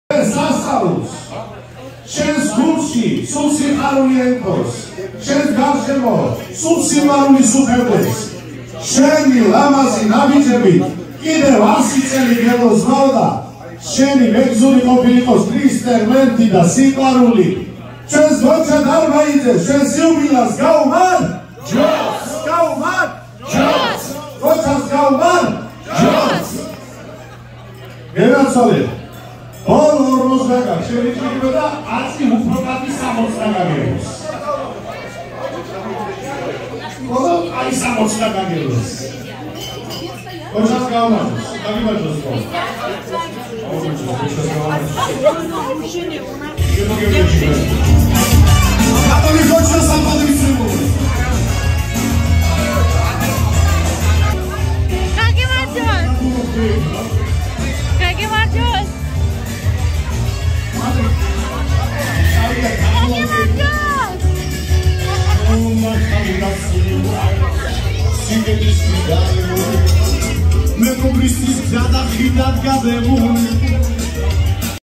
Tamada on Gocha birthday party in Batumi restaurant
Congratulations with birthday to my friend Gotcha, Georgian party Miami, restaurants Gem,Batumi